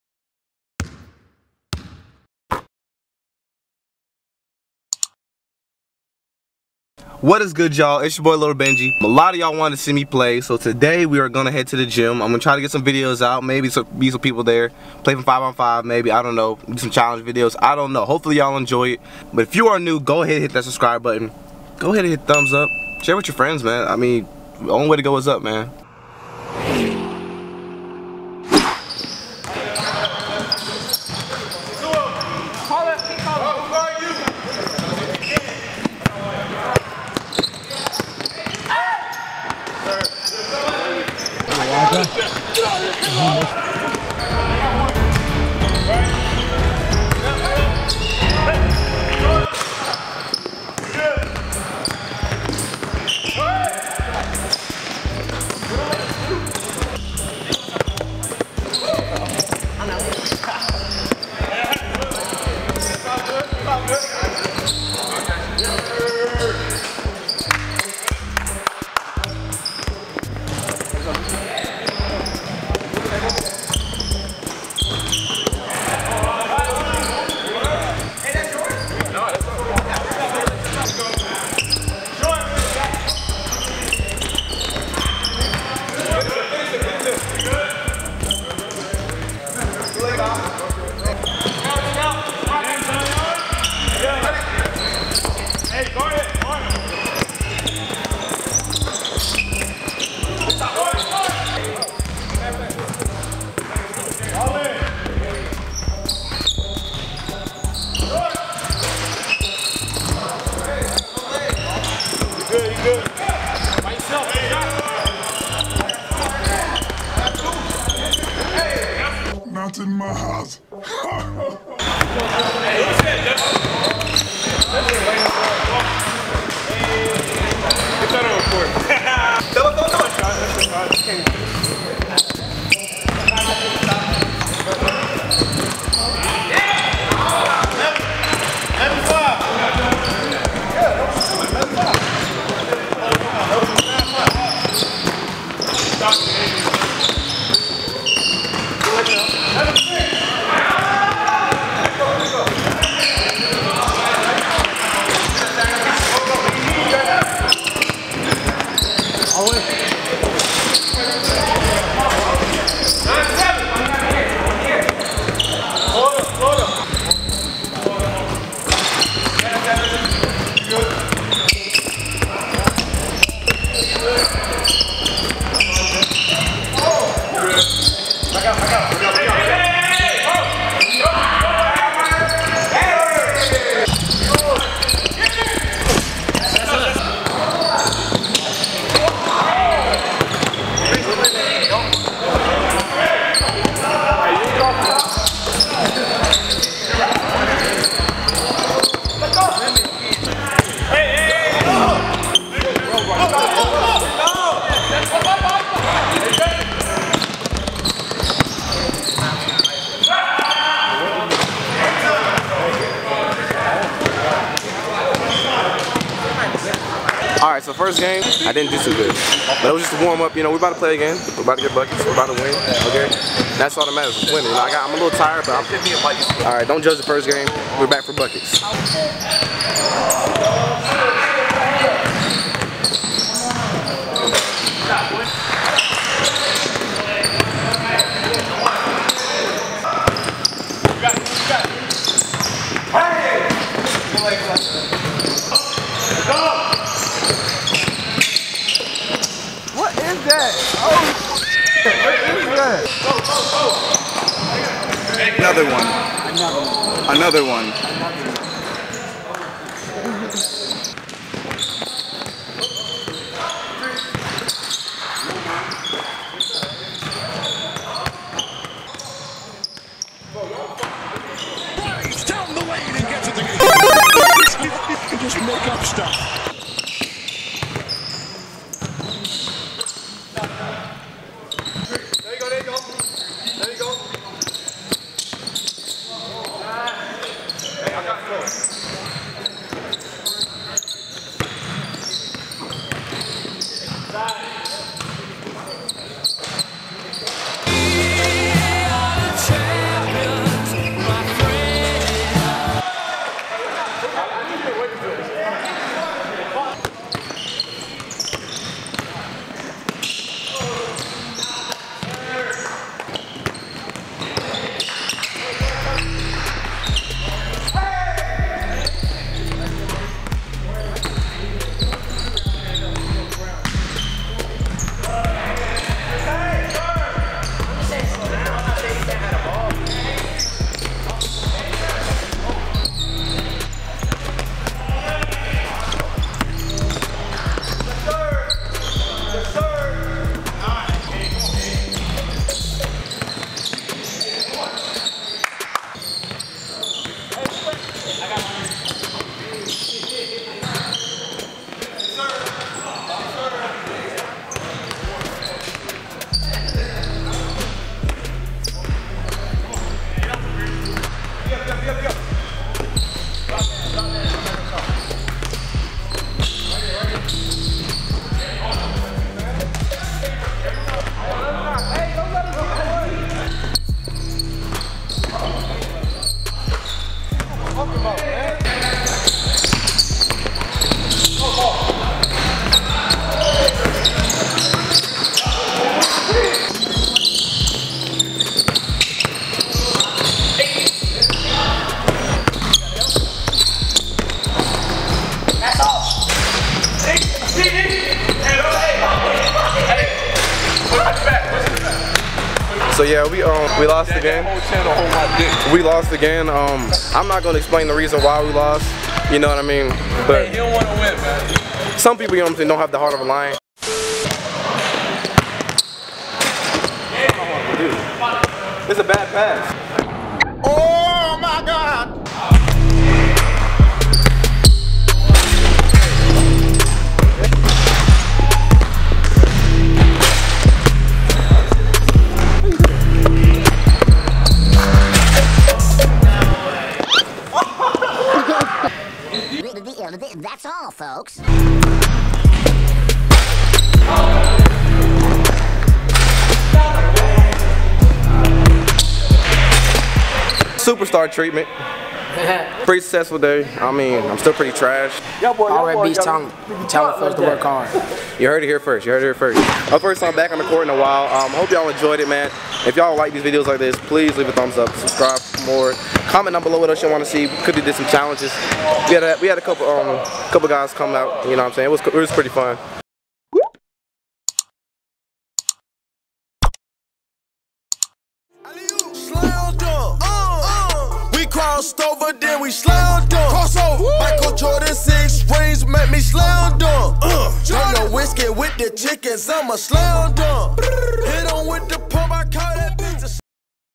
BEST BASKETBALL PLAYER ON YOUTUBE?! IRL DUNK CONTEST w/ D1 PROSPECTS!
The time has arrived for Lil Benji to save IRL basketball! Thanks to everyone who requested this video! Thanks to everyone involved including, 2019 Sam Okauru, 2018 Virginia Tech commit, Jarren McAllister, Kayode (Ky) Oke-Bello, and MORE! Let us know if you want to see more videos like this! (P.S. I'm EXTREMELY RUSTY)

FOLLOW LIL BENJI!